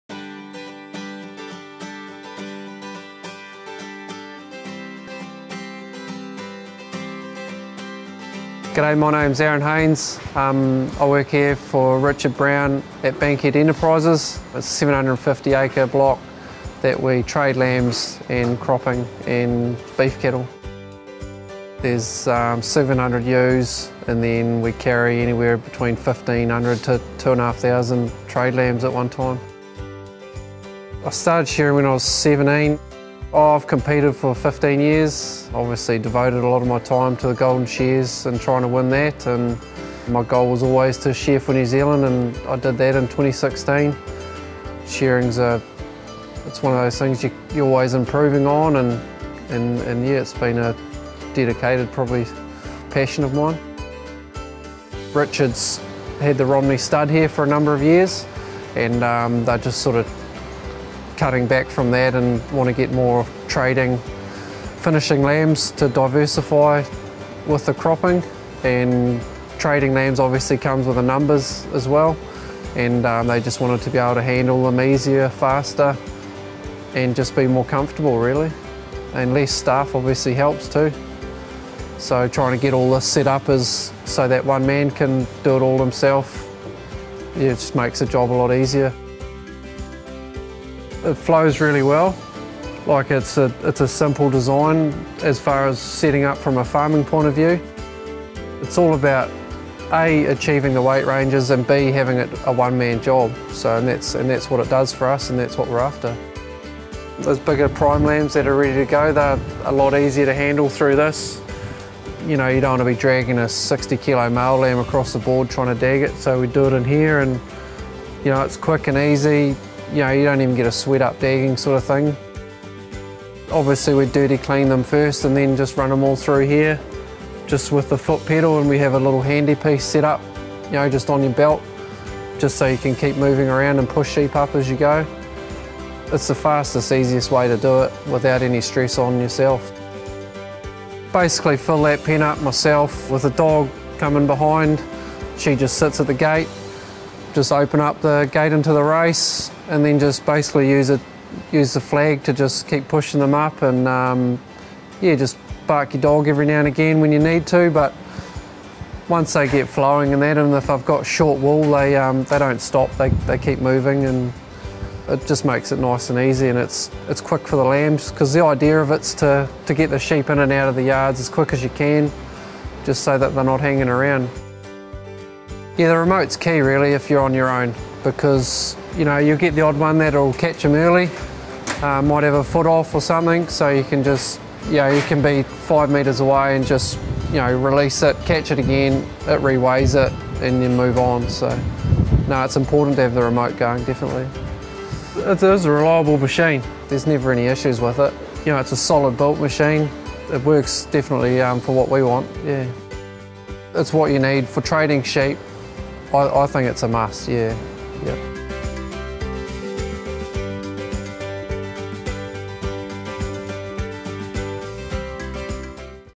A Must for Lamb Traders | Aaron Haynes talks the Racewell HD4
The Racewell Sheep Handler is a proven and highly popular, multi-purpose handling system for sheep traders. Listen to Aaron Haynes from Fielding, New Zealand talk about how he's found the Racewell HD4 for their lamb trading operation. Weighing, drafting, crunching and more, it's all done in the Racewell, and is only a one-person task.

If you'd like to learn more about the Racewell HD3 Sheep Handler, please get in touch with our team.